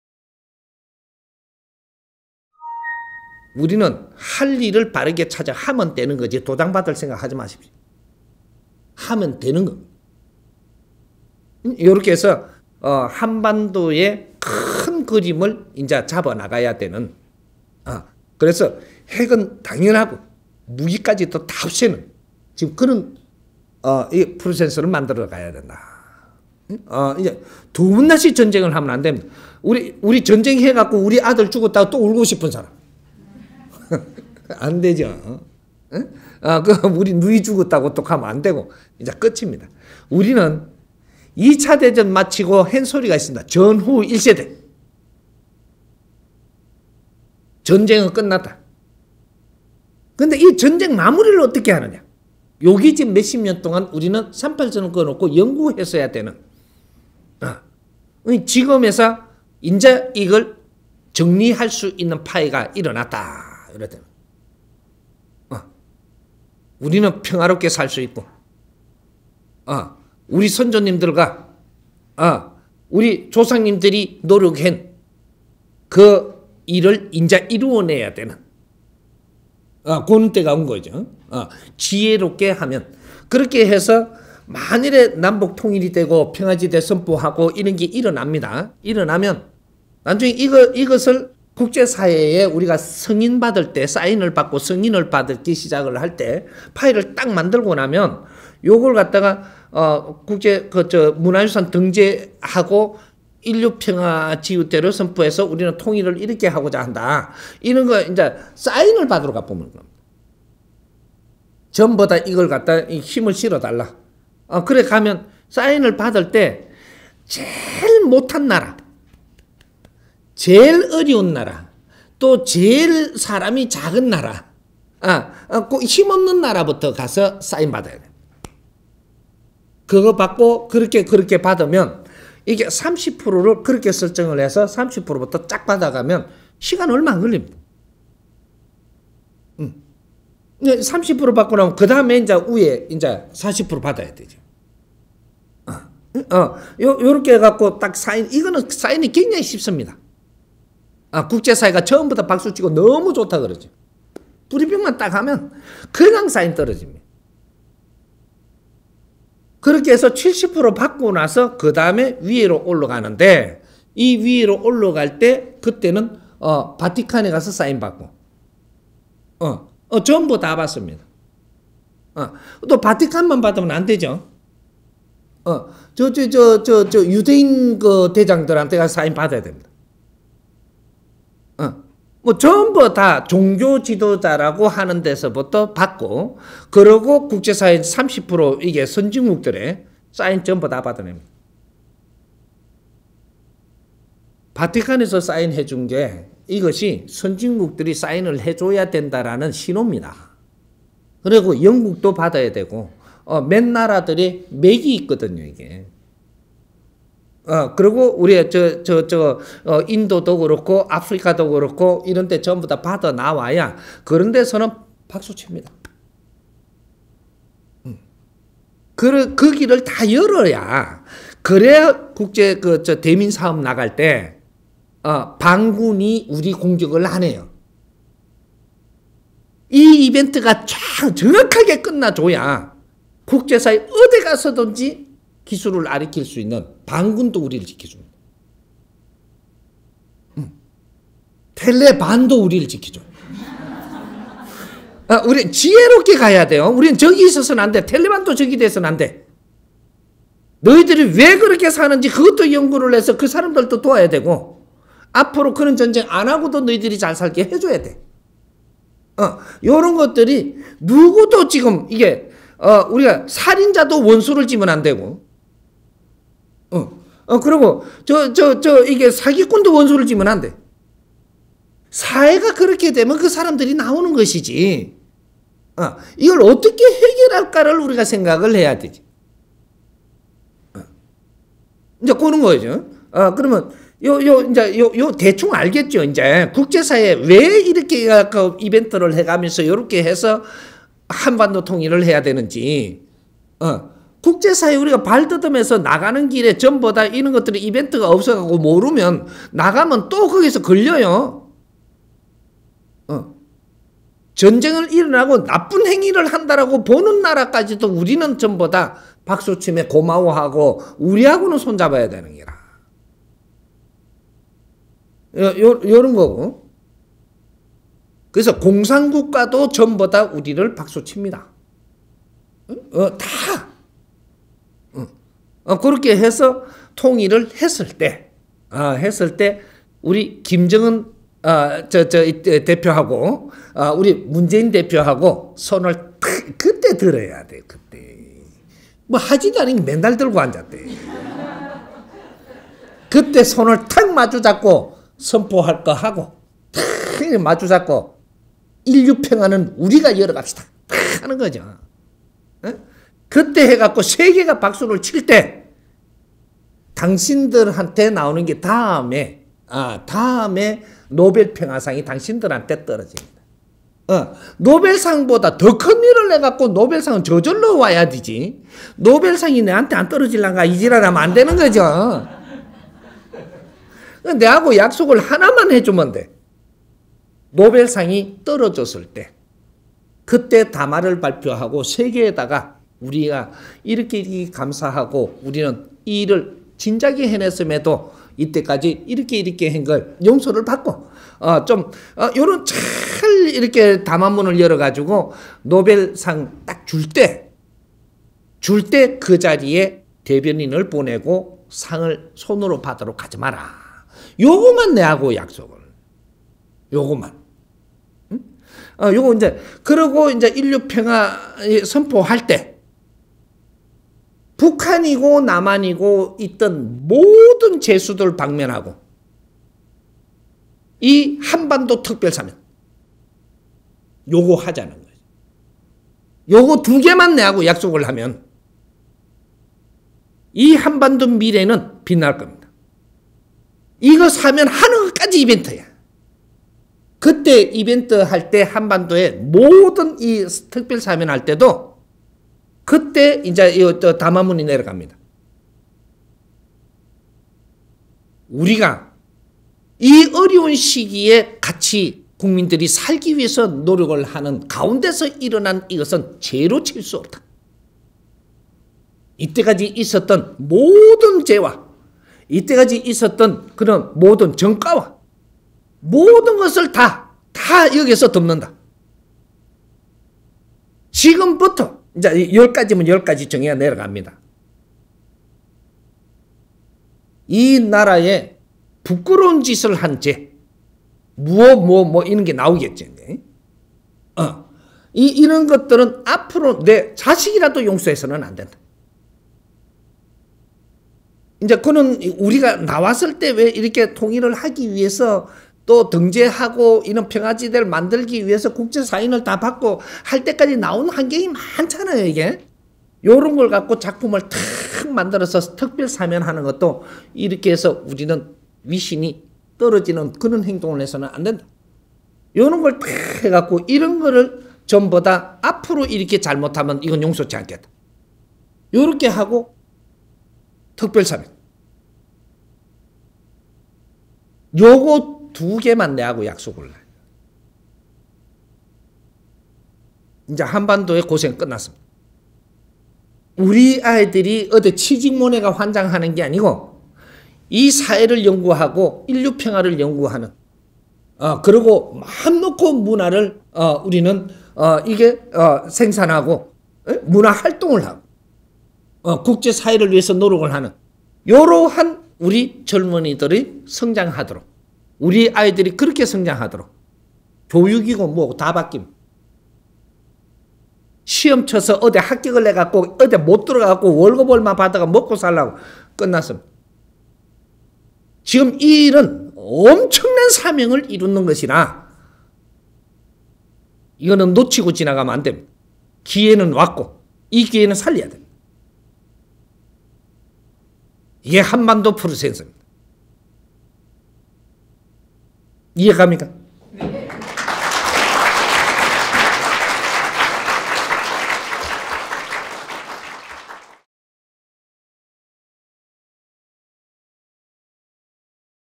[특별포럼][통일] 7966강 김정은 위원장의 신의 한 수 - 한반도 프로세스(5_5)
질문 : 방금 스승님의 말씀을 들어보니까 모든 칼자루는 실제로 김정은 위원장이 다 가지고 있는 것 같습니다. 이즈음에서 저희가 감히 여쭙는다면 김정은 위원장의 신의 한 수가 필요할 것 같습니다. 그 신의 한 수는 저희가 연구를 해야 한다지만 이 시간에 조금이나마 풀어주시면 감사하겠습니다.

강의 일자 : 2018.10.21 서울 / 정법시대 : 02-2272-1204